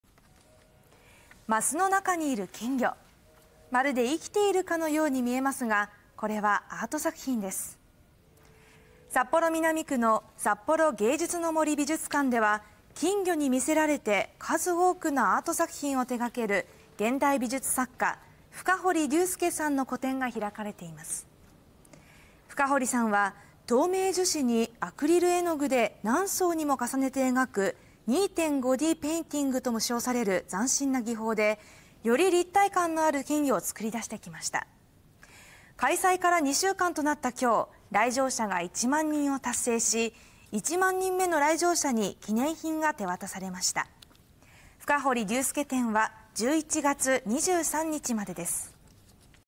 まるで生きているかのような立体感のある金魚アート 「深堀隆介展」札幌芸術の森美術館で開催から2週間で"来場者1万人"達成 (23/09/29 18:03)
札幌芸術の森美術館で開かれている「深堀隆介展」の来場者が1万人を達成し、記念のセレモニーが行われました。

　マスのなかにいる金魚。まるで生きているかのように見えますが、これはアート作品です。

　札幌市南区の札幌芸術の森美術館では金魚に魅せられて数多くのアート作品を手掛ける現代美術作家深堀隆介さんの個展が開かれています。

　深堀さんは、透明樹脂にアクリル絵具で何層にも重ねて描く「2.5Dペインティング」とも称される斬新な技法でより立体感のある金魚を作り出してきました。

　開催から2週間となった9月29日、来場者が1万人を達成し、1万人目の来場者に記念品が手渡されました。

　深堀隆介展は11月23日までです。